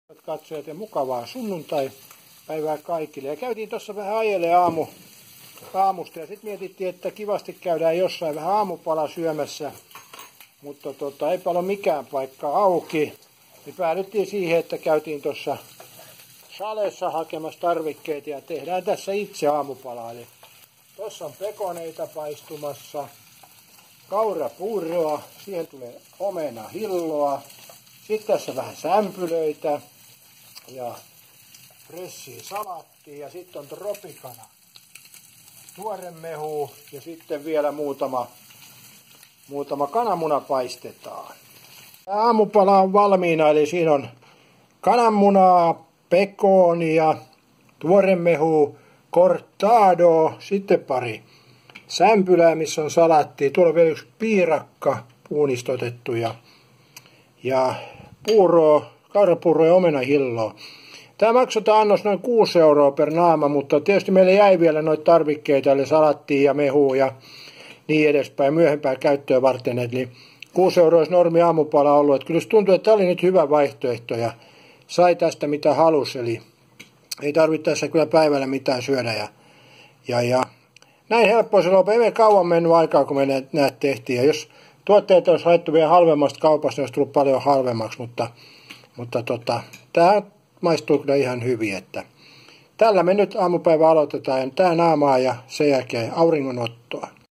Aamupala hintaan 3,5 - 4€ (Kalliista kaupasta)
Hinta tarkistettiin ja todettiin kaikkine kuvioineen noi 3,5-4 euron hintaiseksi.